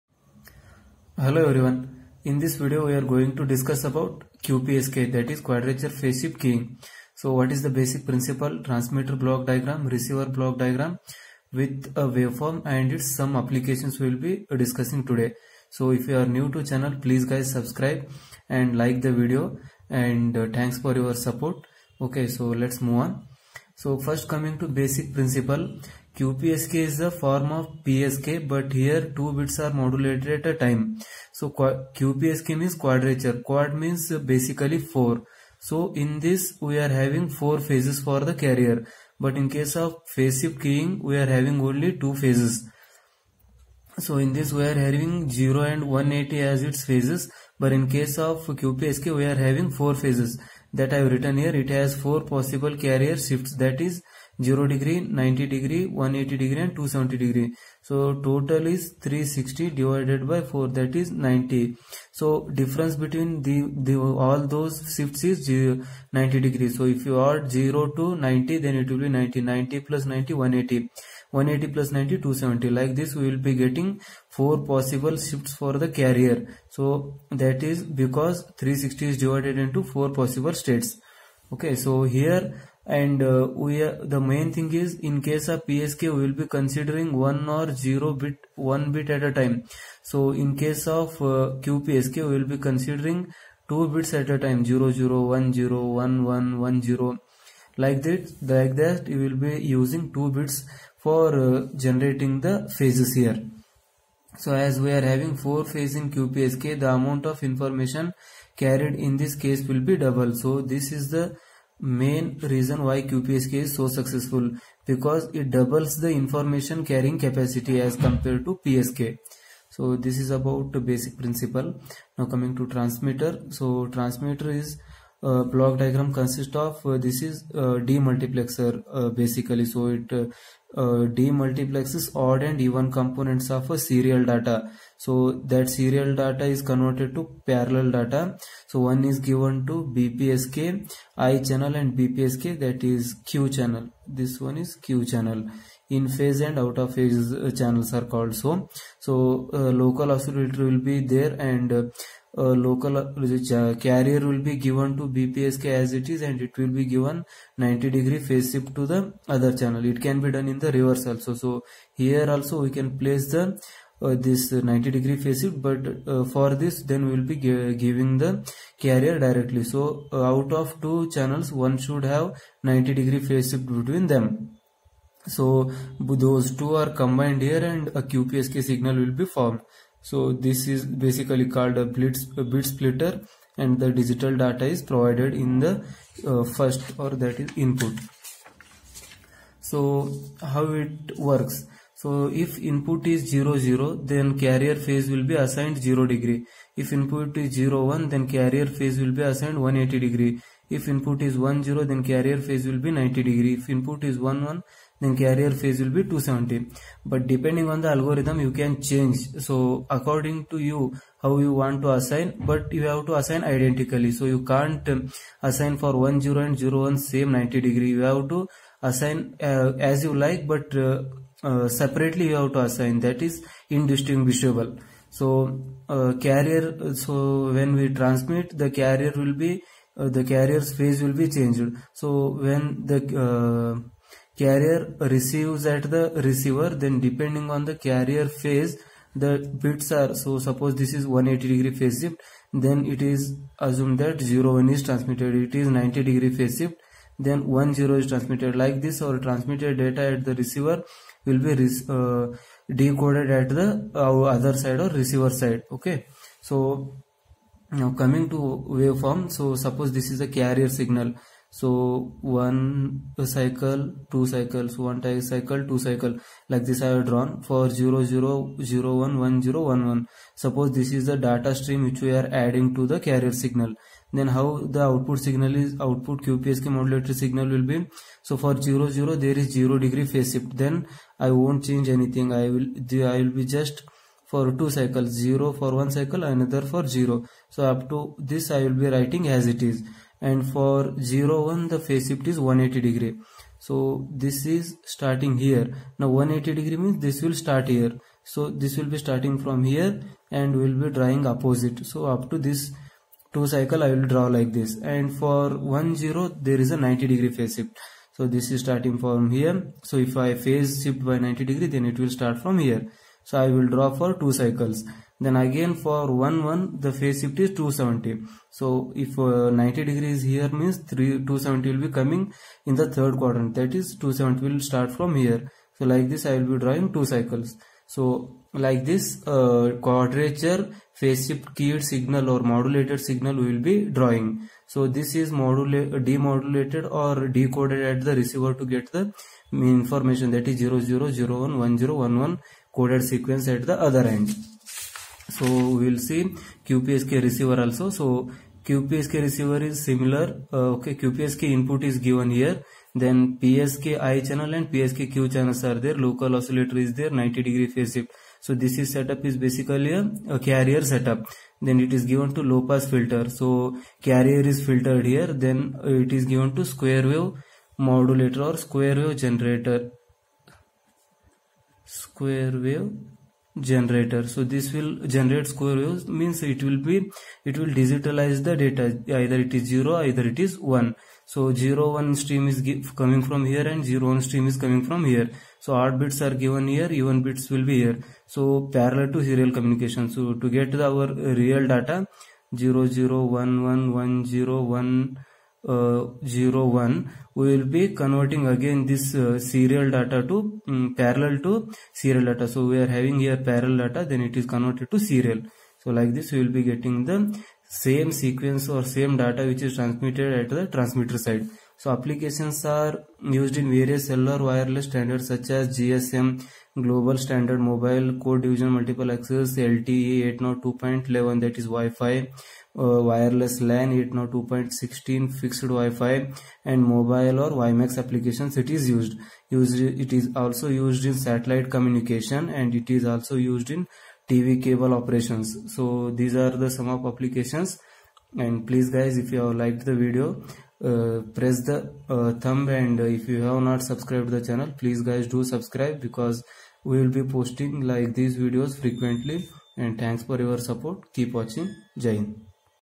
quadracture phase shift keying ( QPSK ) in digital communication / what is QPSK , with its waveform
This video what is the meaning of qpsk and its related block diagrams of transmitter and receiver with waveforms .